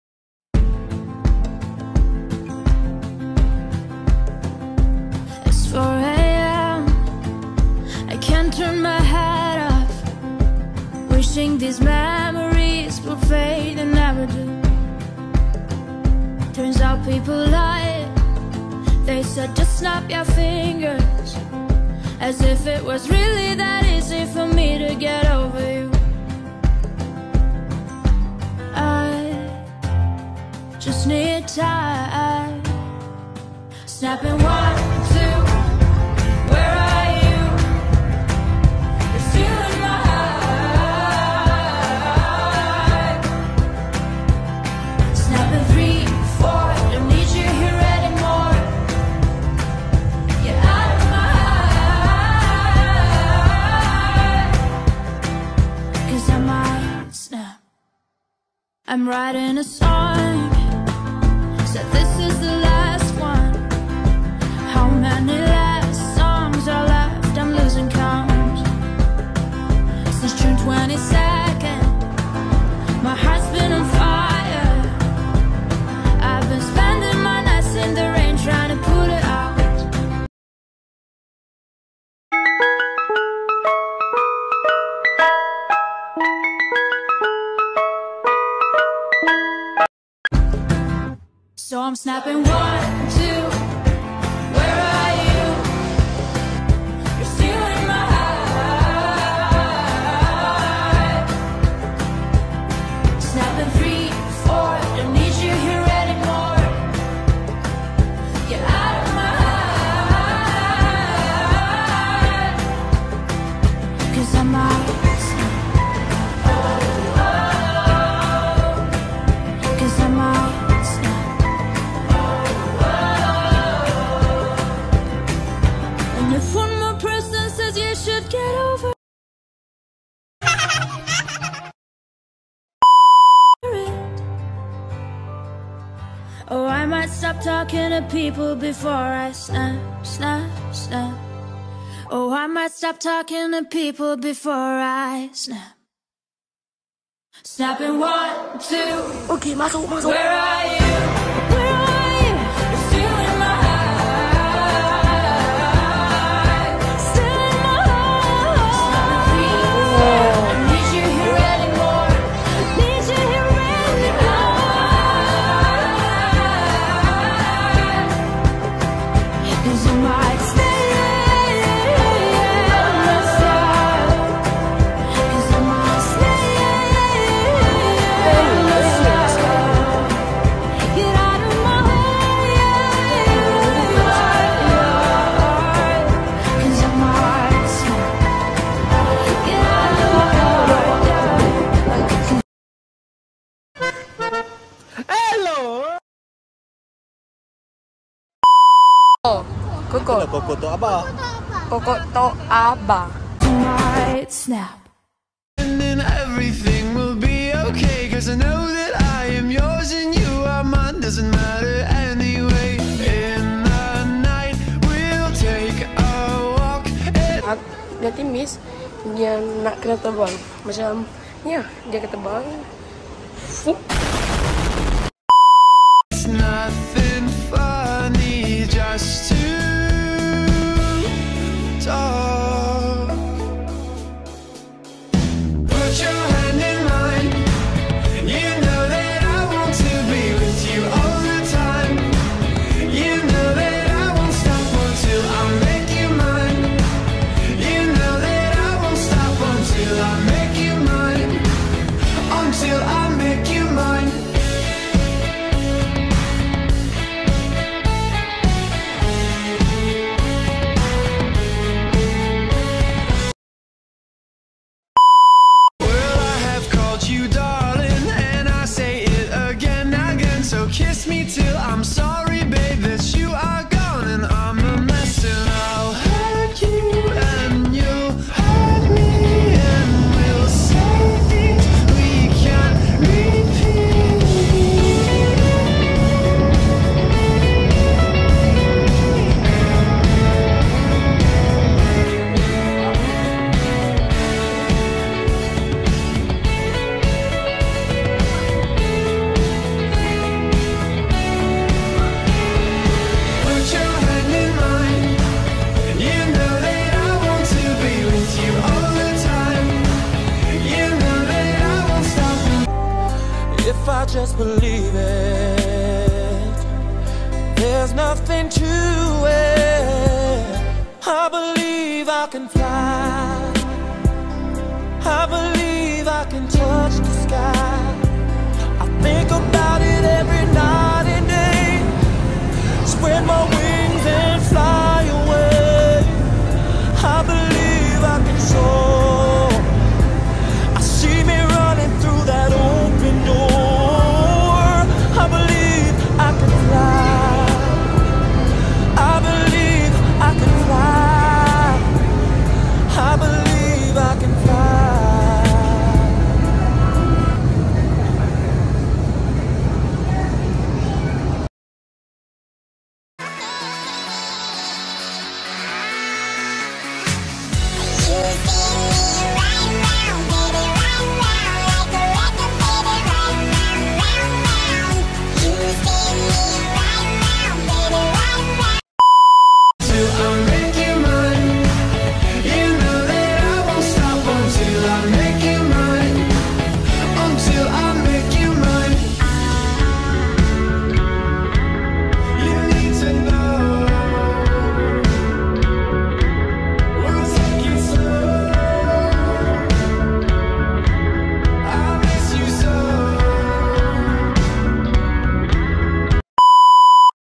Windlab Indoor Skydiving kat 1Utama.. Terbaik lah!!! (VLOG)
Memang best Pengalaman Miz Menjadi Superman😂😂😂🤣🤣🤣 korang pun patut cuba guys!

BYEEEE

ASSALAMUALAIKUM